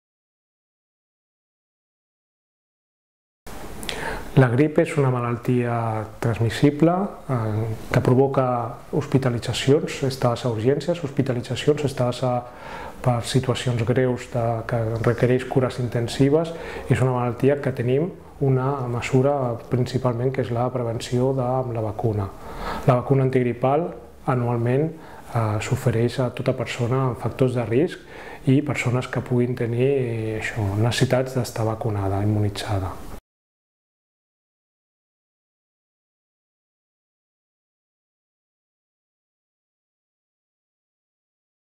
La grip pot ser una malaltia molt greu
Toni López, infermer i líder de promoció de la salut d ela Gerència Territorial de Barcelona, fa recomanacions sobre la vacunació de la grip.